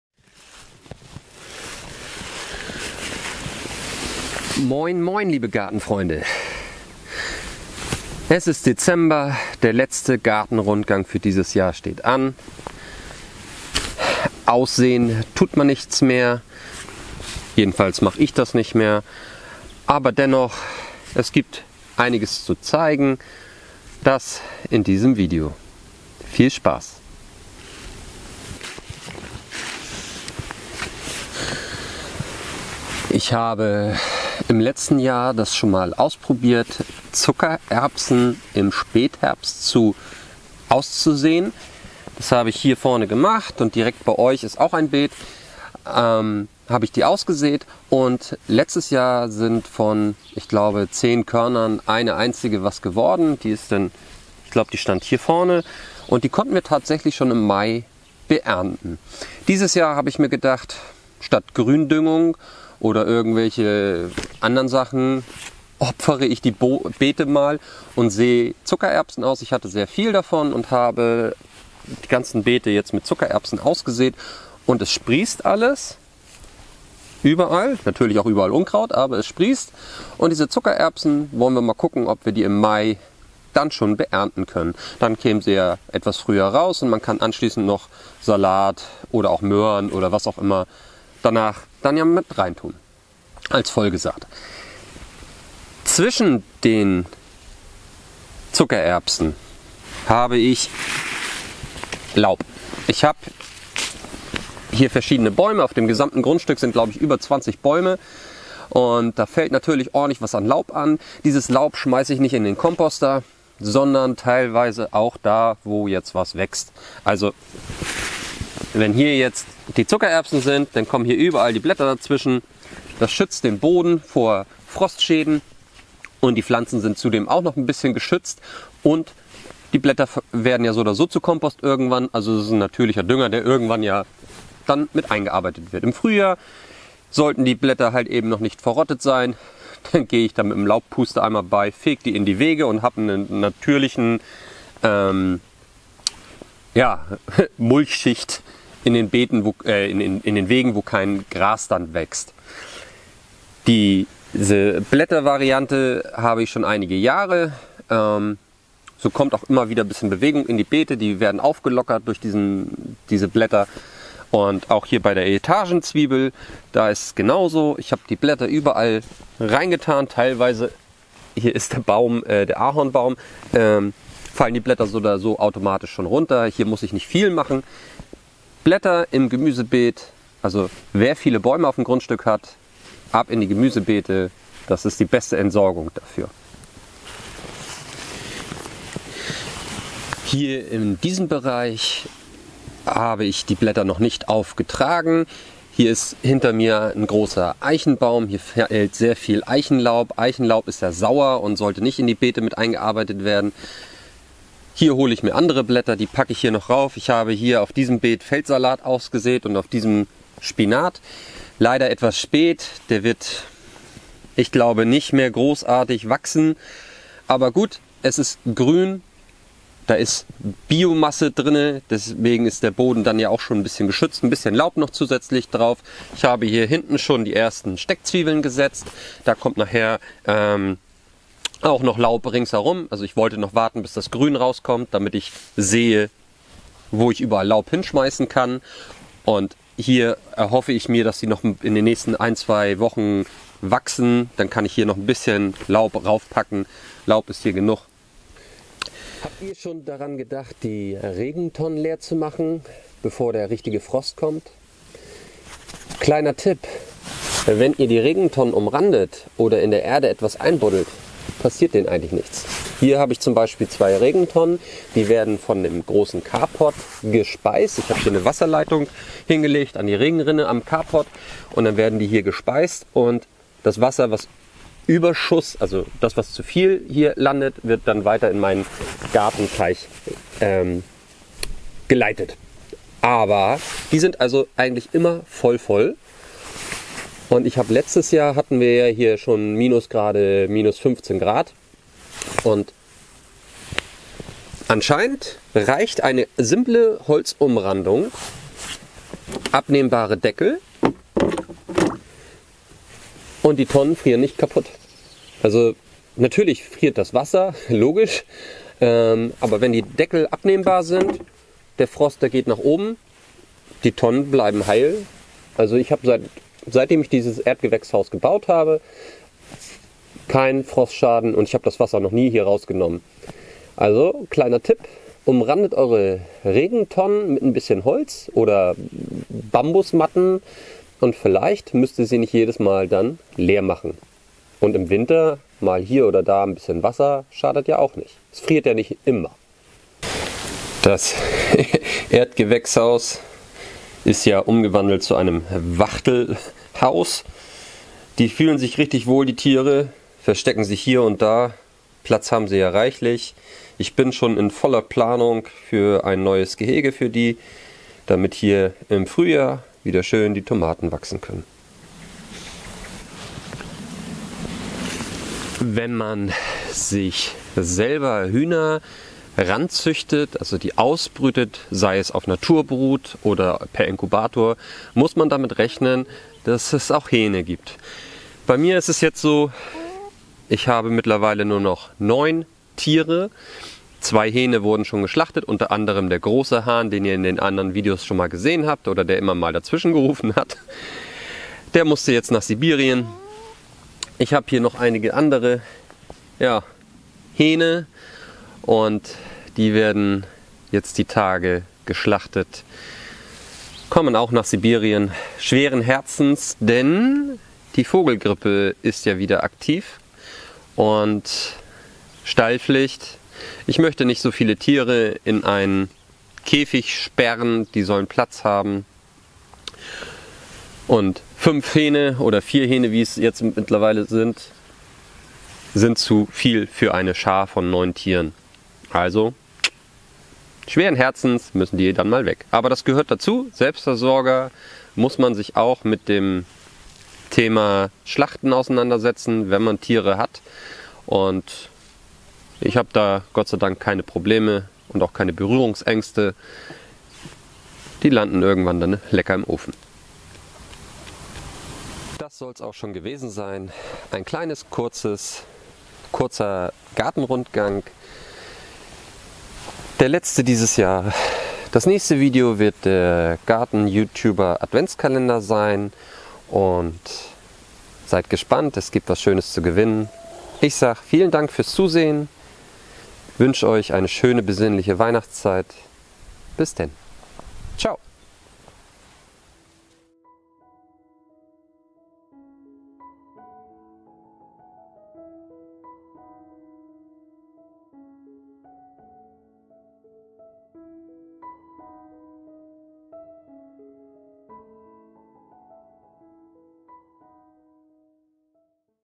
ein Gartenrundgang im Dezember 2021
Kleiner Gartenrundgang mit ein paar Tipps und Tricks vom Gartenpapa.

Alte historische Gemüsesorten-Sortiment

Gemüsesamen Sortiment
Der Selbstversorger
Der Selbstversorger

bekomme ich eine kleine Provision.
Der Einkauf kostet euch keinen Cent mehr.

Meine Wunschliste

Liebe Grüße
Stefan der Gartenpapa